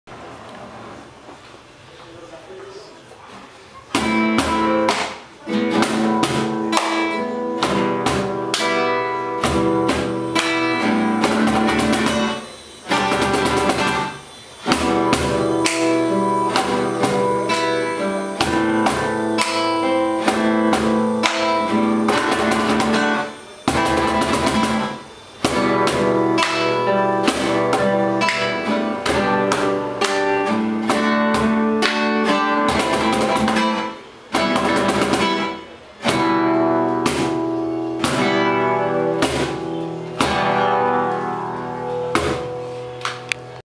TALLER MUSICAL EN CASA 2013 - Nicolas Gomez (Ensamble II) Con caja peruana.
Caja peruana y palmas acompañada de guitarra (Ejercicio corto)

Octubre 5 del 2013